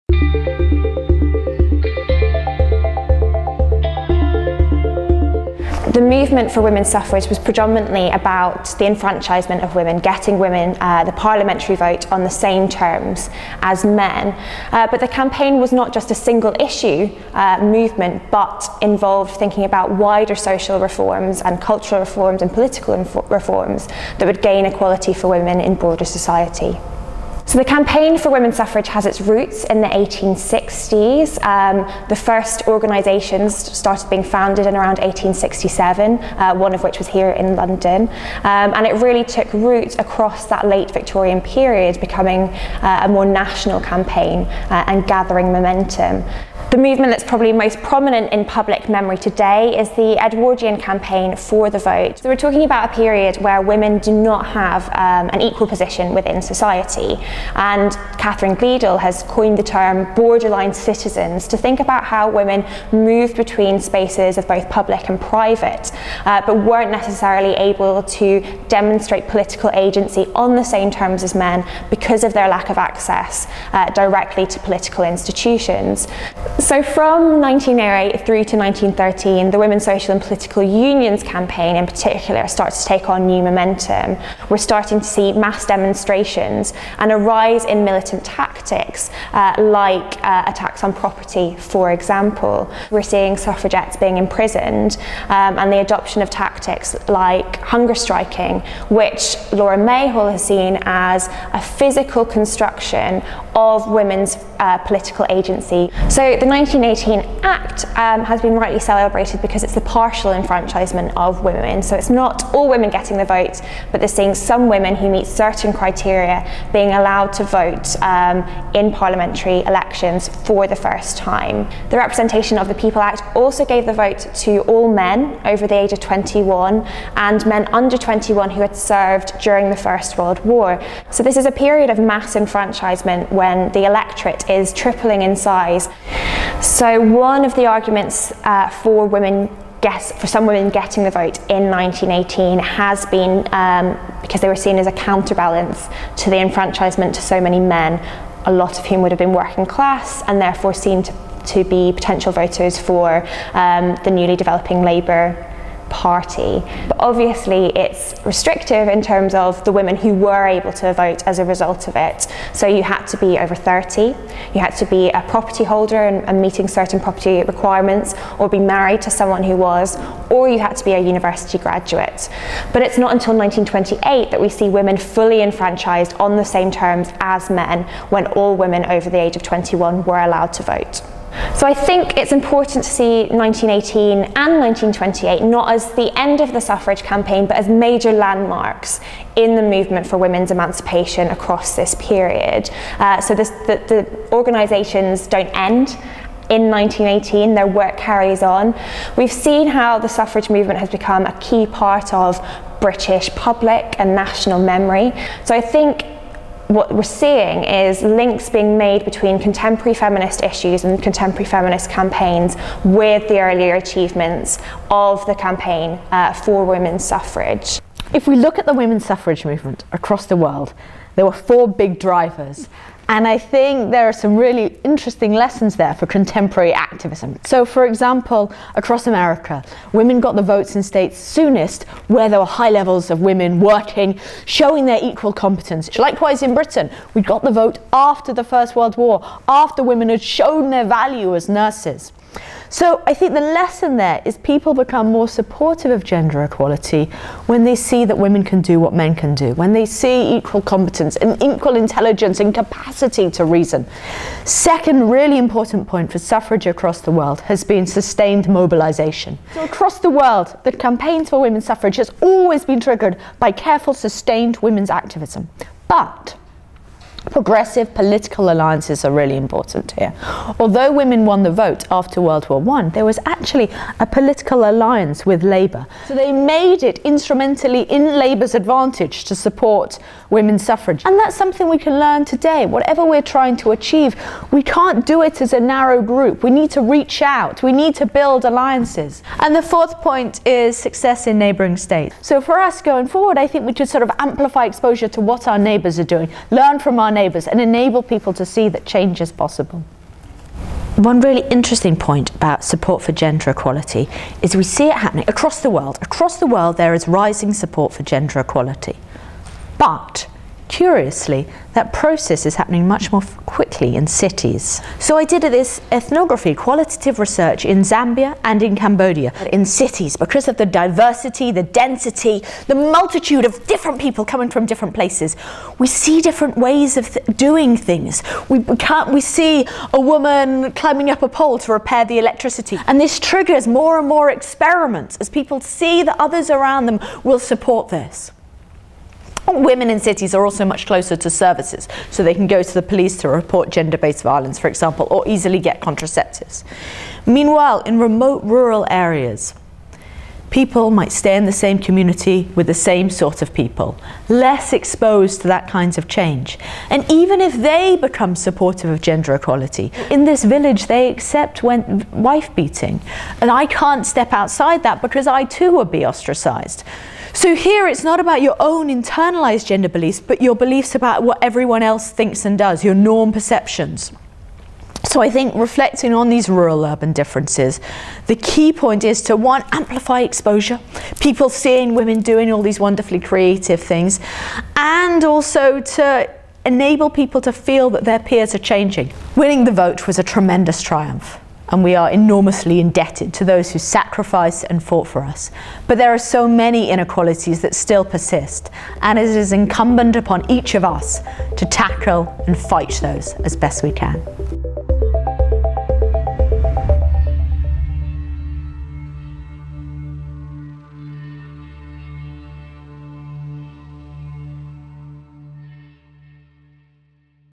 Votes for women: the history of women's suffrage and lessons for today
Dr Anna Maguire and Dr Alice Evans talk about the history of the women's suffrage movement and its relevance for gender equality today.

Dr Anna Maguire, History, discusses key milestones and events in the suffrage campaign and explains how the movement began gathered momentum in the 1860s and throughout the Victorian period. The Representation of the People Act in 1918 marked a significant moment - although the Act only allowed a select group of women to vote.

Dr Alice Evans discusses the 4 key drivers of the movement across the world, examining the importance of seeing women in employment, social mobilisation to show people not to be despondent because change is possible, political alliances and success in neighbouring states. Dr Evans explains how this relates to her research in to 'norm perceptions'.

Credits

Images:
Museum of London
The Women's Library - LSE
Imperial War Museum